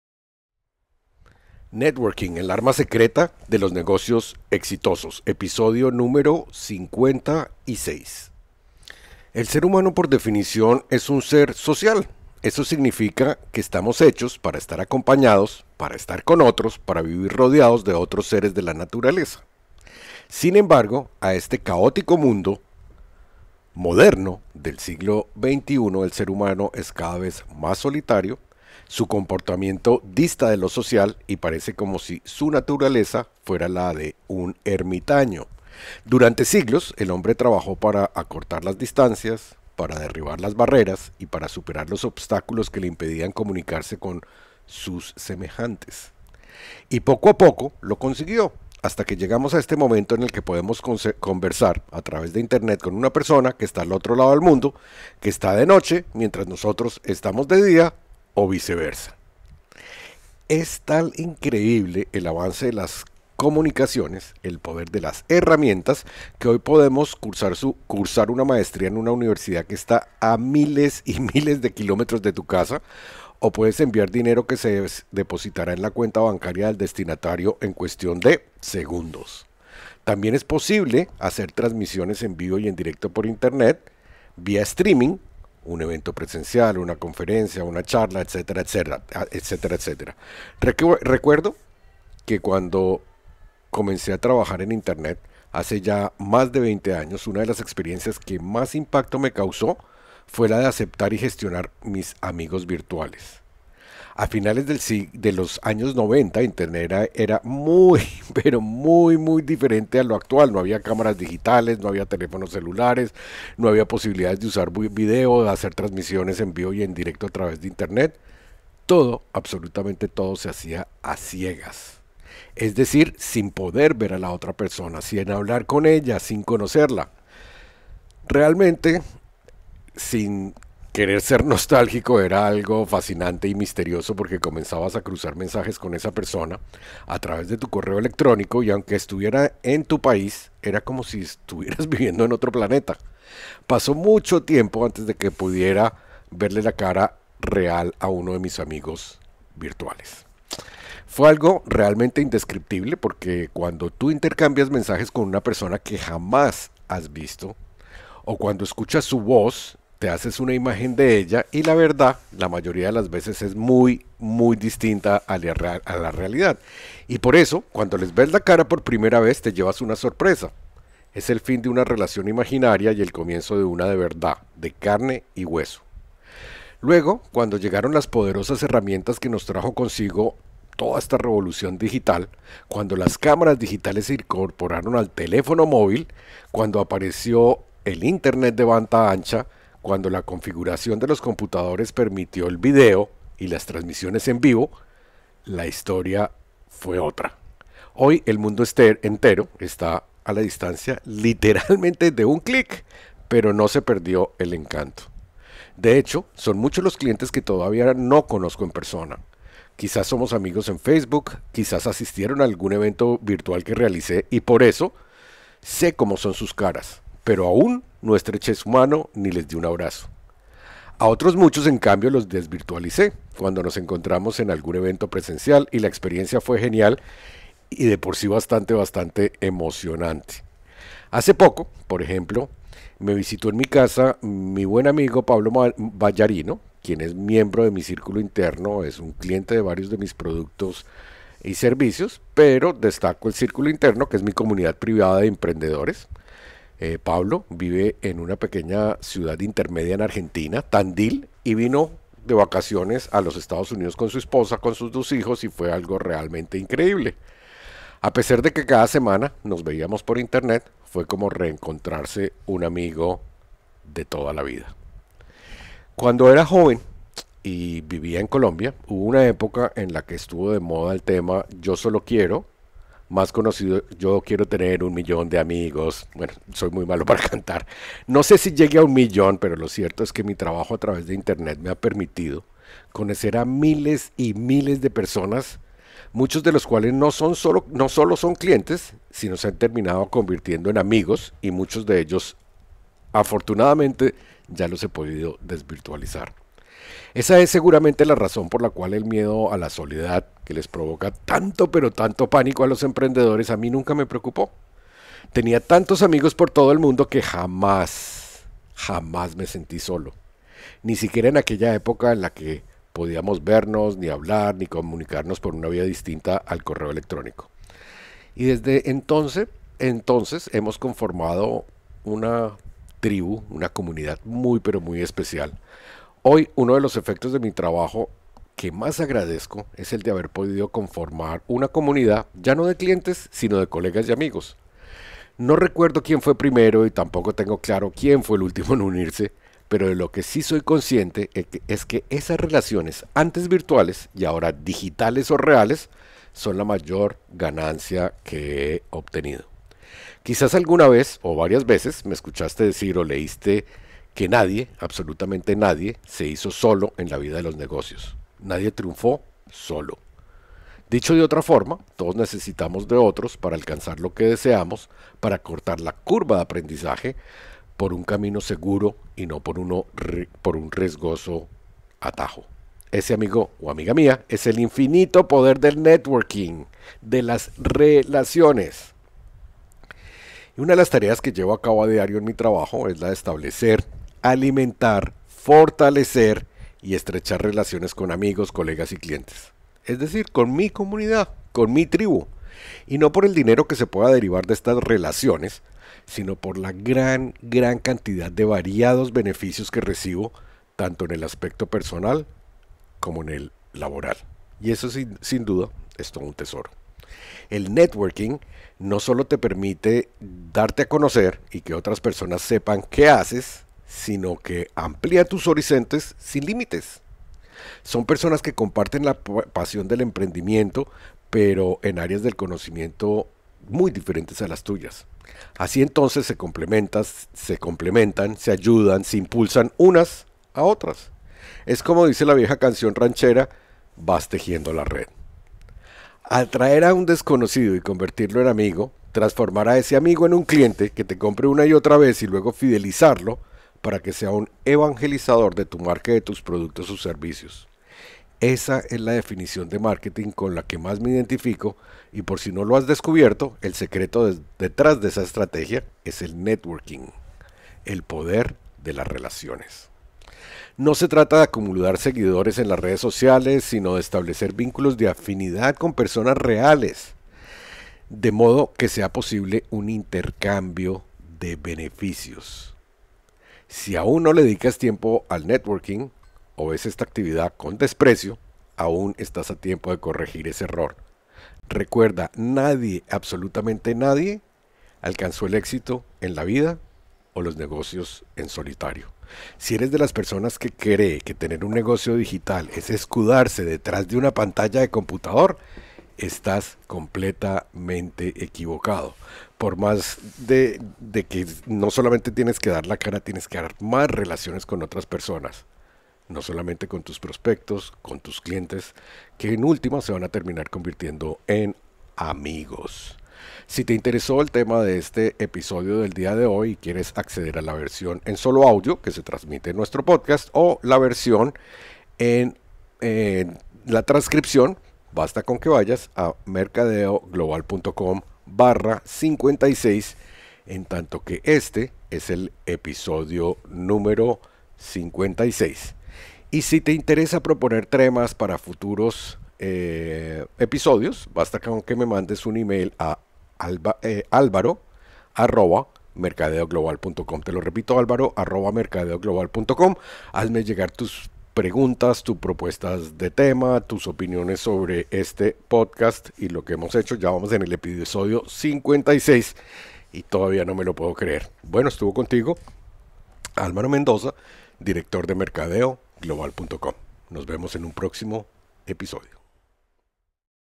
Networking, el arma secreta de los negocios exitosos
El miedo a estar solo, que tanto atormenta a los emprendedores, hoy ya no es un problema si haces uso de una de las estrategias más poderosas que existen: el networking. La finalidad es ayudar a otras personas y apalancarte en el conocimiento y las experiencias de otros, que te permiten potenciar fortalezas y minimizar debilidades. Relaciones reales en escenarios virtuales.